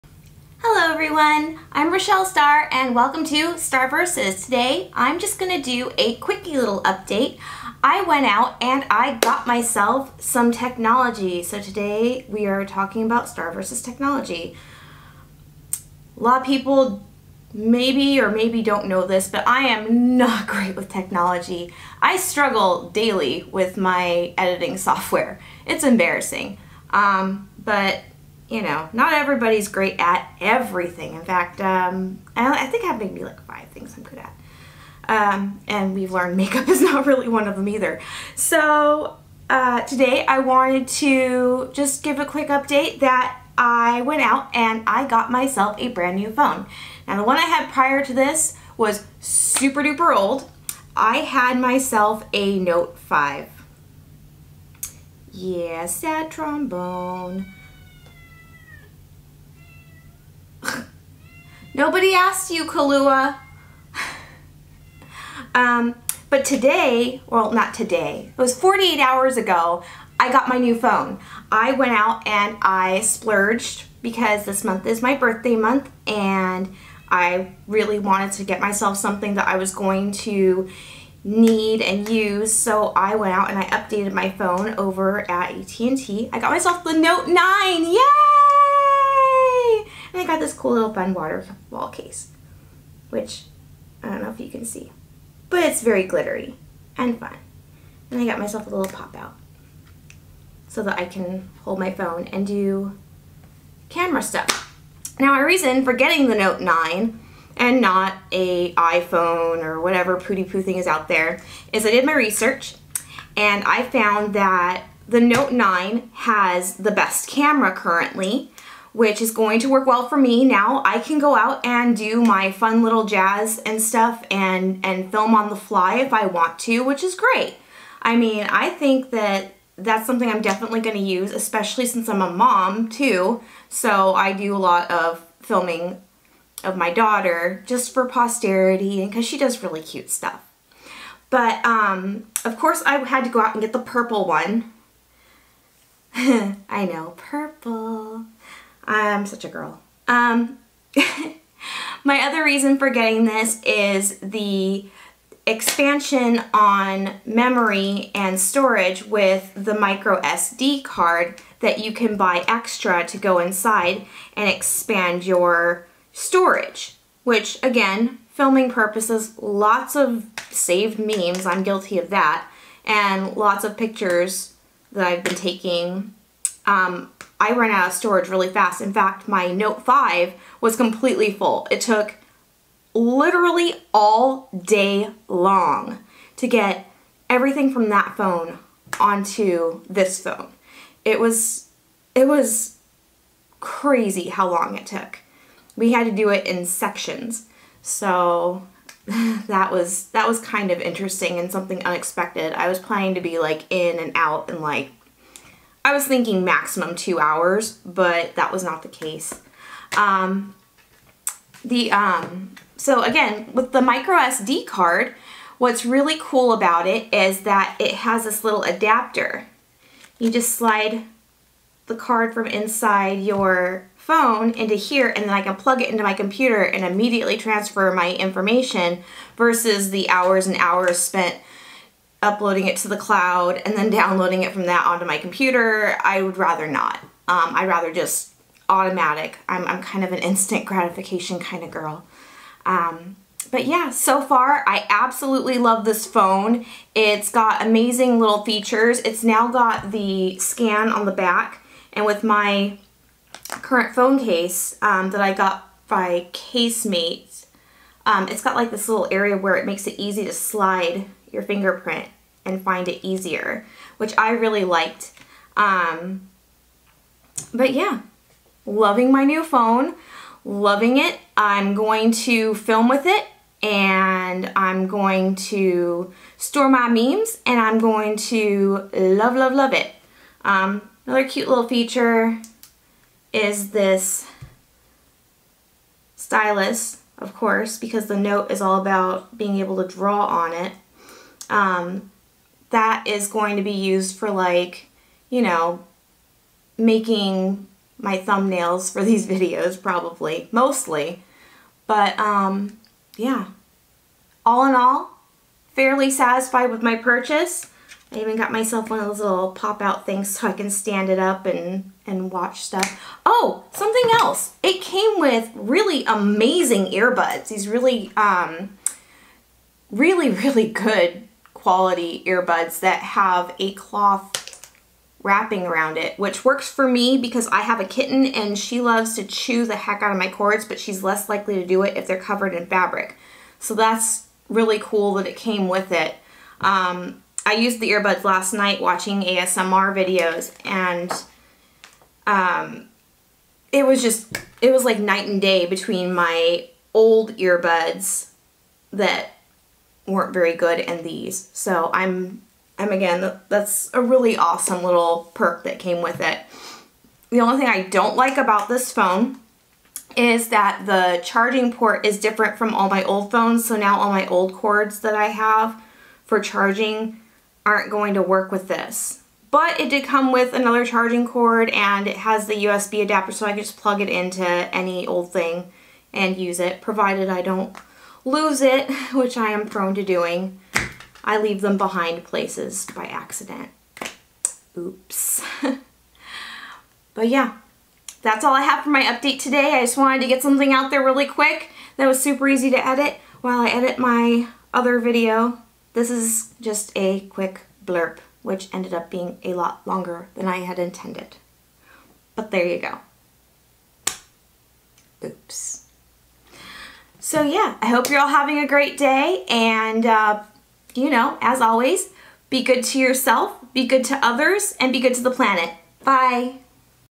Starr versus Technology! I bought the new Samsung Galaxy Note 9
Just my quick update for the week. I bought the newest and best phone for my needs, which is the Samsung Galaxy Note 9. First impressions & why I picked this phone.